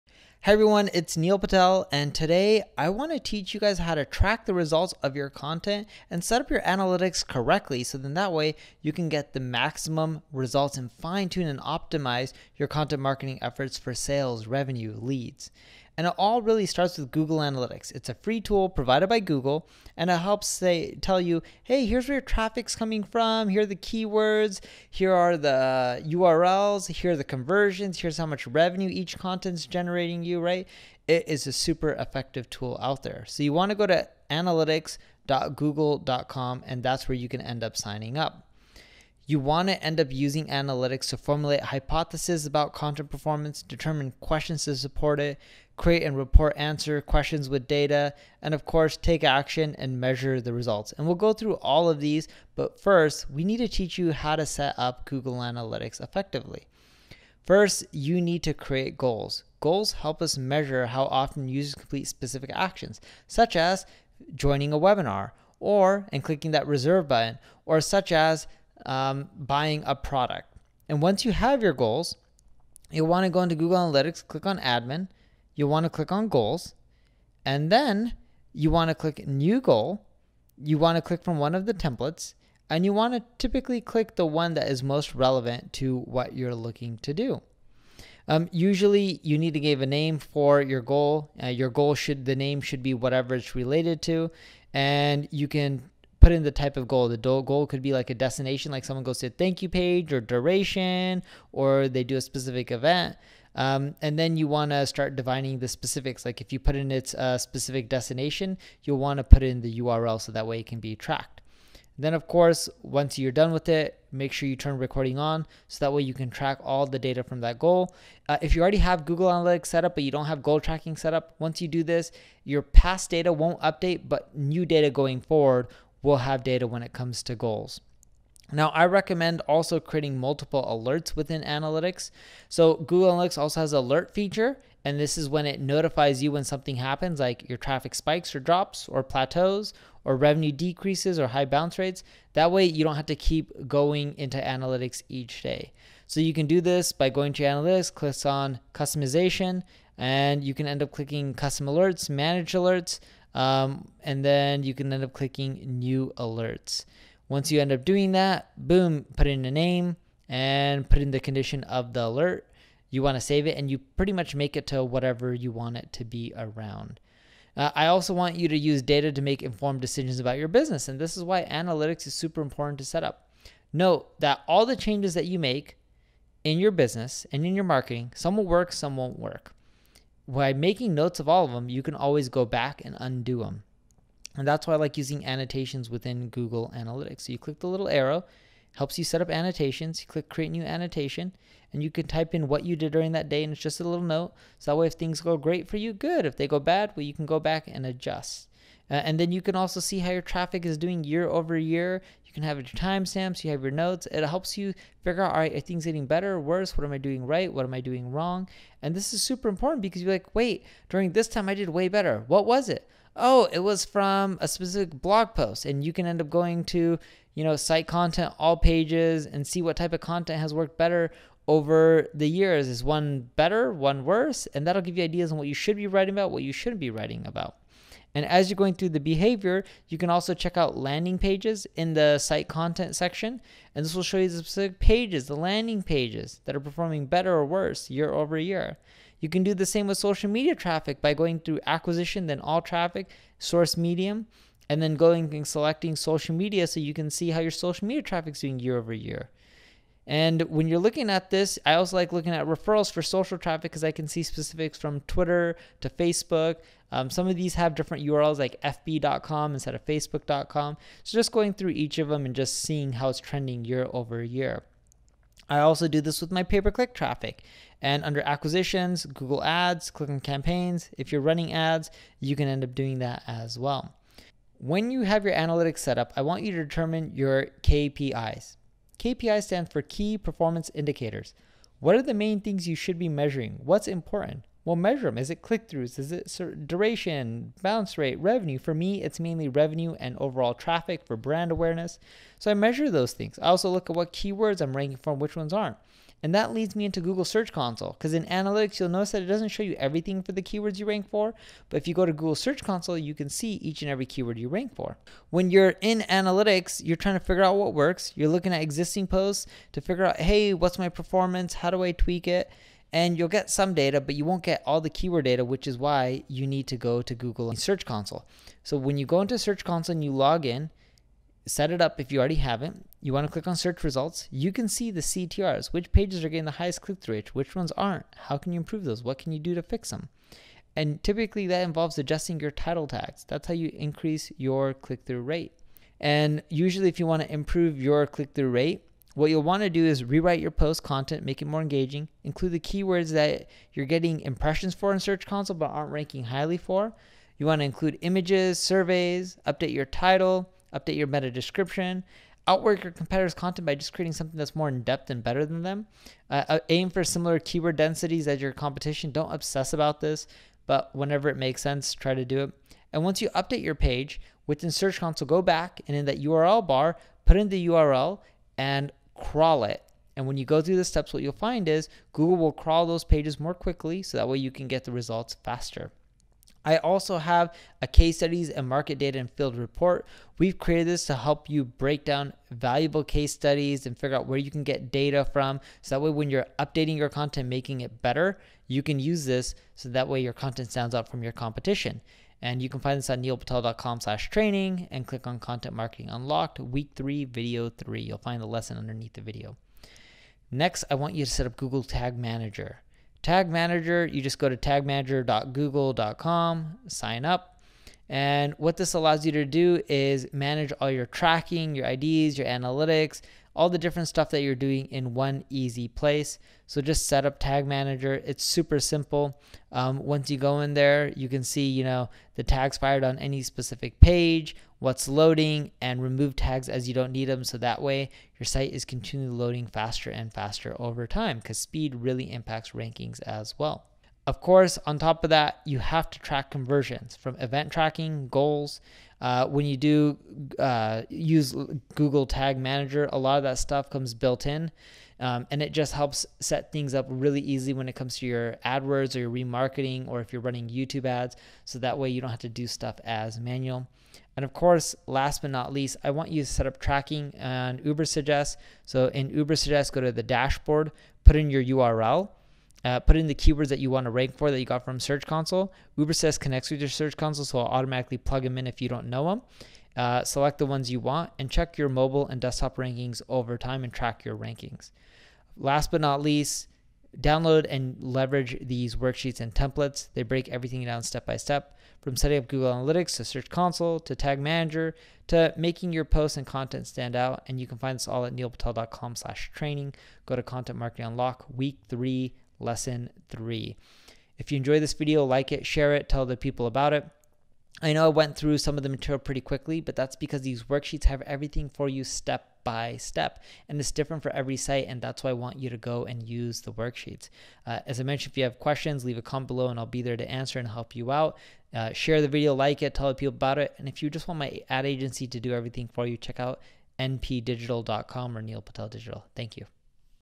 Tools To Improve Your Content Marketing - Module 3 - Lesson 3 - Content Marketing Unlocked
Today I want to teach you guys how to track the results of your content and set up your analytics correctly. So then that way you can get the maximum results and fine tune and optimize your content marketing efforts for sales, revenue, leads.

And it all really starts with Google Analytics. It is a super effective tool out there. So you want to go to analytics.google.com and that's where you can end up signing up. You want to end up using analytics to formulate hypothesis about content performance, determine questions to support it, create a report to answer questions with data. And of course, take action and measure the results. And we'll go through all of these, but first we need to teach you how to set up Google Analytics effectively.

First, you need to create goals. Goals help us measure how often users complete specific actions. And once you have your goals, you want to go into Google Analytics, click on Admin. You want to click on Goals, and then you want to click New Goal. You want to click from one of the templates and you want to typically click the one that is most relevant to what you're looking to do.

Usually you need to give a name for your goal. And then you want to start defining the specifics. Like if you put in, it's a specific destination, you'll want to put it in the URL. So that way it can be tracked.

And then of course, once you're done with it, make sure you turn Recording on, so that way you can track all the data from that goal. If you already have Google Analytics set up, but you don't have goal tracking set up, once you do this, your past data won't update, but new data going forward will have data when it comes to goals.

Now I recommend also creating multiple alerts within analytics. So Google Analytics also has a alert feature. And this is when it notifies you when something happens like, your traffic spikes or drops, or plateaus, or revenue decreases or high bounce rates.

You want to save it, and you pretty much make it to whatever you want it to be around. I also want you to use data to make informed decisions about your business. And this is why analytics is super important to set up.

So you click the little arrow, helps you set up annotations. Click Create New Annotation, and you can type in what you did during that day, and it's just a little note. So if things go great for you, good. If they go bad, well you can go back and adjust.

So you can see how your social media traffic's doing year over year. And when you're looking at this, I also like looking at Referrals for social traffic, cause I can see specifics from Twitter to Facebook.

And that leads me into Google Search Console. So when you go into Search Console and you log in, set it up, if you already haven't. You want to click on search results.

That's how you increase your click through rate. And usually if you want to improve your click through rate, what you'll want to do is rewrite your post content, make it more engaging, include the keywords that you're getting impressions for in Search Console, but aren't ranking highly for. You want to include images, surveys, update your title, update your meta description.

Next, I want you to set up Google Tag Manager. And what this allows you to do is manage all your tracking, your IDs, your analytics, all the different stuff that you're doing in one easy place. So just set up Tag Manager. It's super simple.

Of course, on top of that, you have to track conversions, from event tracking goals. When you do use Google Tag Manager, a lot of that stuff comes built in and it just helps set things up really easy when it comes to your AdWords or your remarketing, or if you're running YouTube ads. So that way you don't have to do stuff as manual.

And of course, last but not least, I want you to set up tracking and Ubersuggest. So in Ubersuggest go to the dashboard, put in your URL, put in the keywords that you want to rank for, that you got from Search Console.

Ubersuggest connects with your Search Console, so it'll automatically plug them in if you don't know them. Select the ones you want and check your mobile and desktop rankings over time and track your rankings.